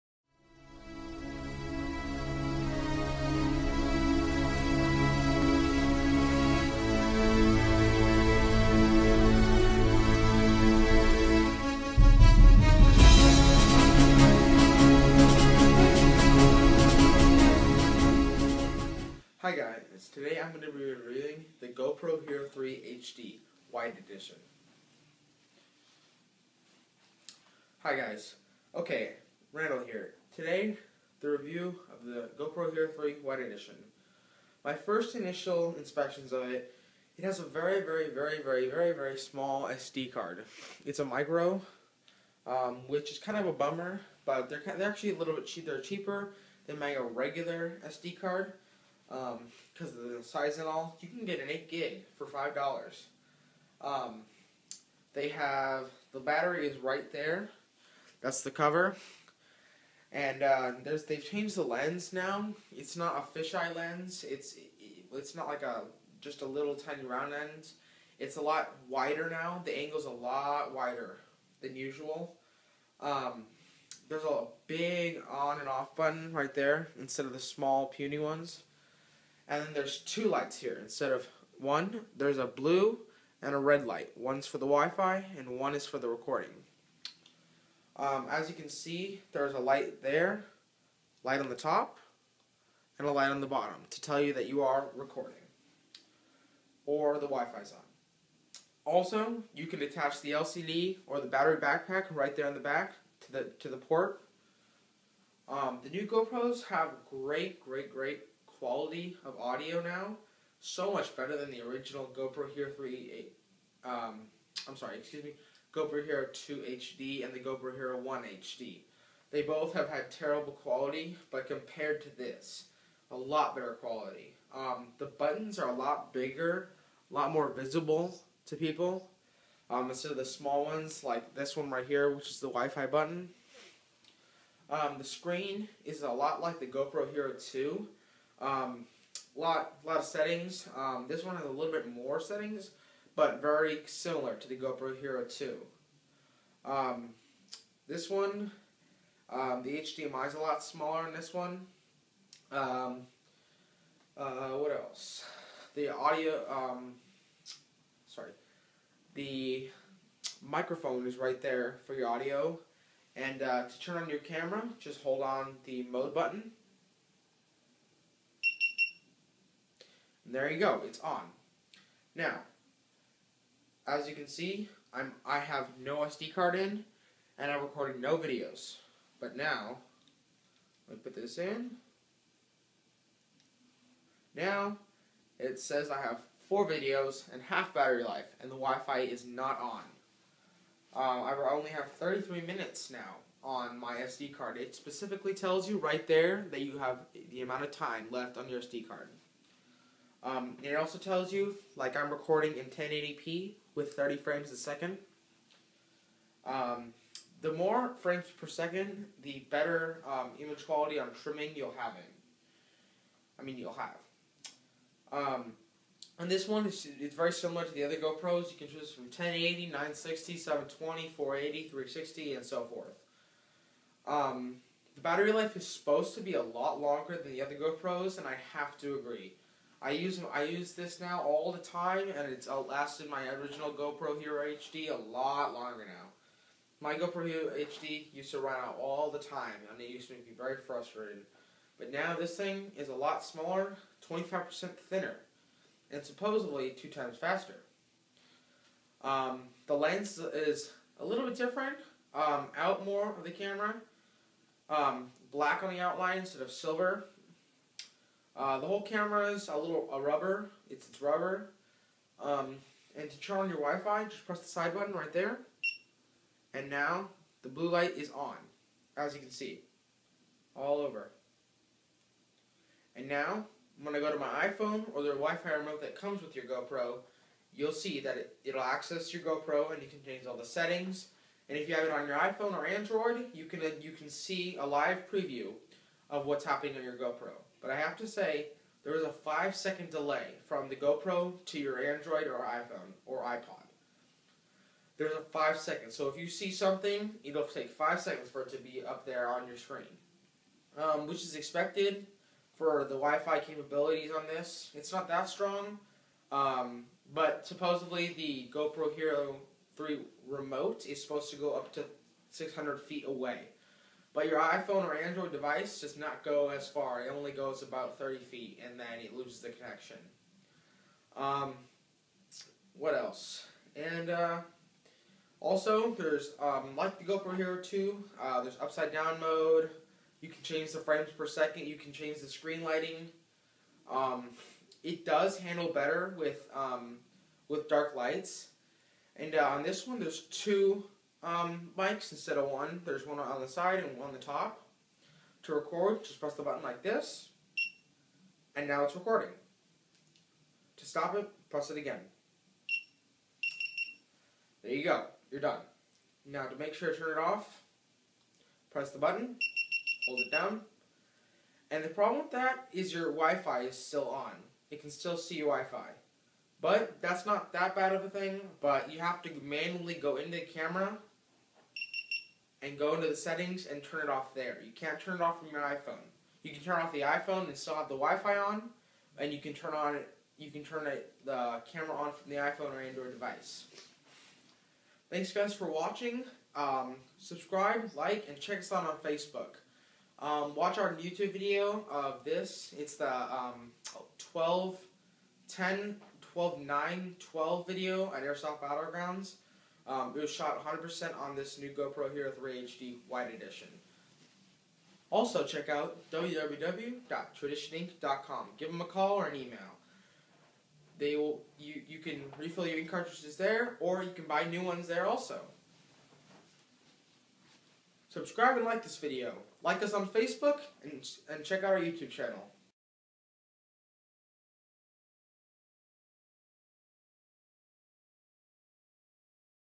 GoPro Hero 3 HD White Edition
This is a REVIEW of the GoPro Hero 3 HD White Edition!
Check out a test of the GoPro Hero 3 HD!